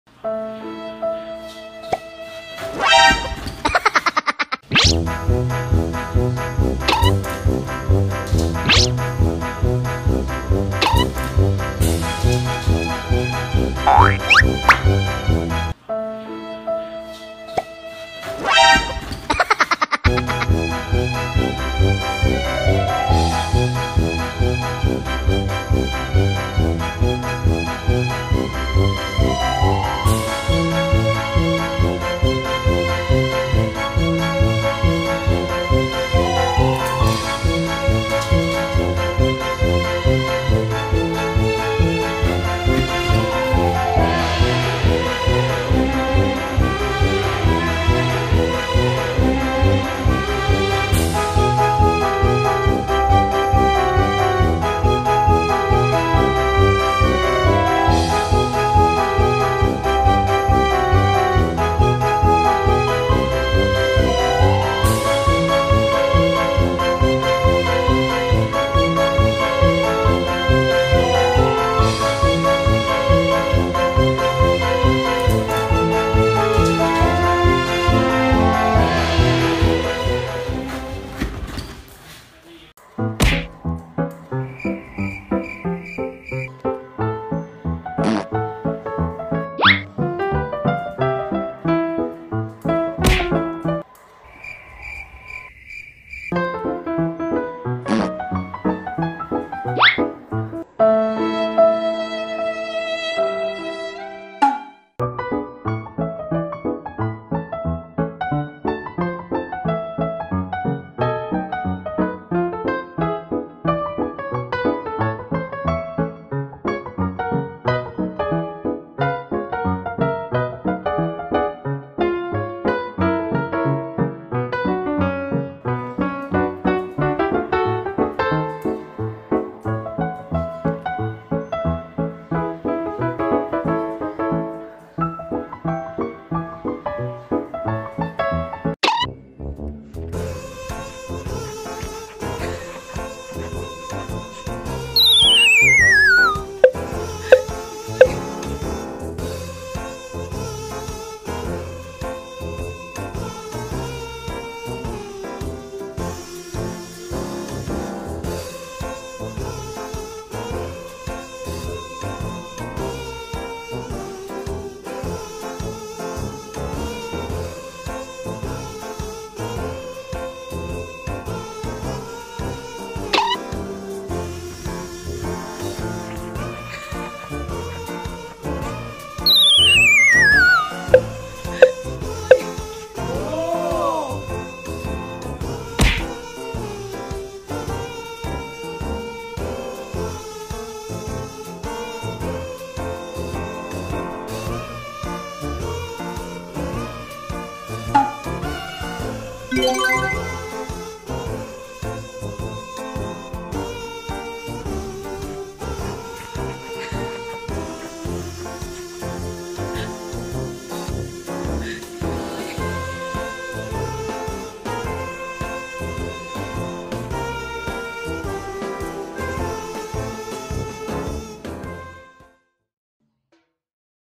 屁柴叼「10入口罩」跑給全家追，馬麻嘆：那是家當｜寵物｜柴犬
☑影片授權：網友 Cherry Tseng